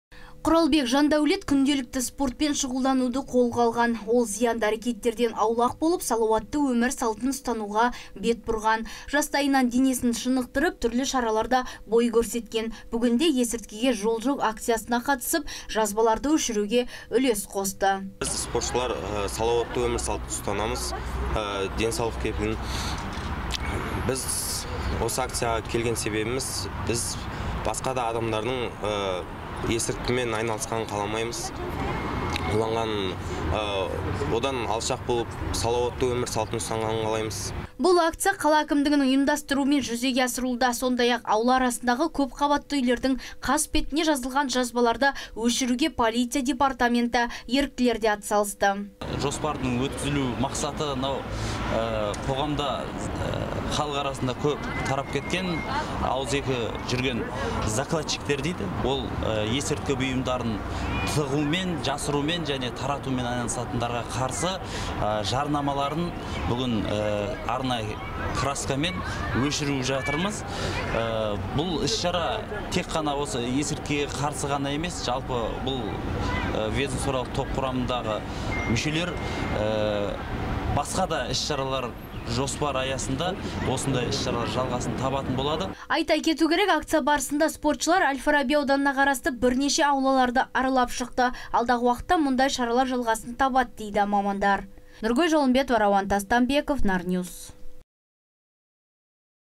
ЕСІРТКІГЕ ЖОЛ ЖОҚ
Есірткіге жол жоқ. П олицияның қызметінде сервистік тәсілді енгізу мақсатында  "Нашақорлыққа қарсы күресте спортшылардың үлесі" атты іс-шара ұйымдастырылды. Мұнда спортшы жастар қала аумағында есірткі сайттары жазылған жазбаларды өшіруге ат салысты. Мақсат, нашақорлыққа қарсы күресті күшейту. Салауатты өмір салтын ұстану.